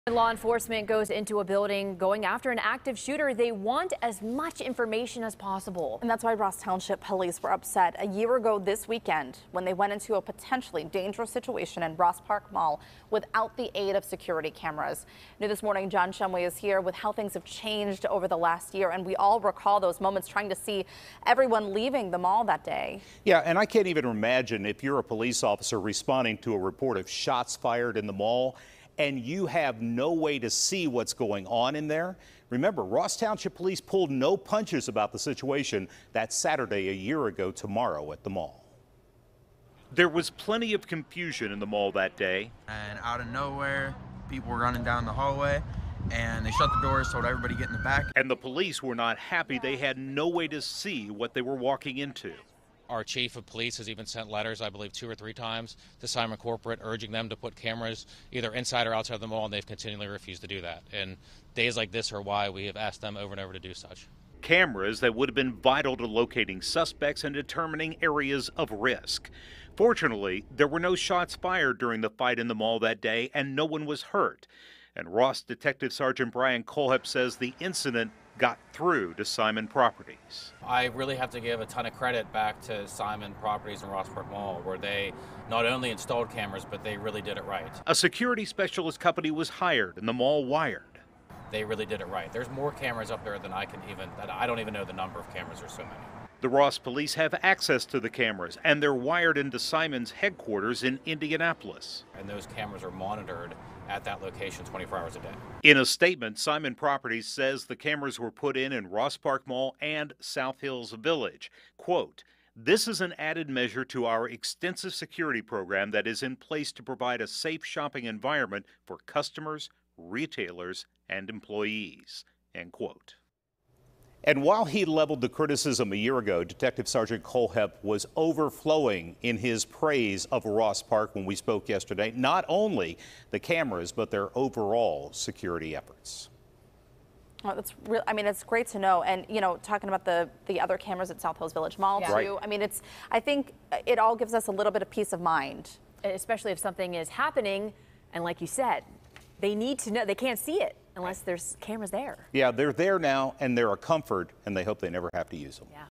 One Year Later: Panic at Ross Park Mall (Pt. 2)
KDKA's John Shumway reports on the incident that caused panic in the North Hills.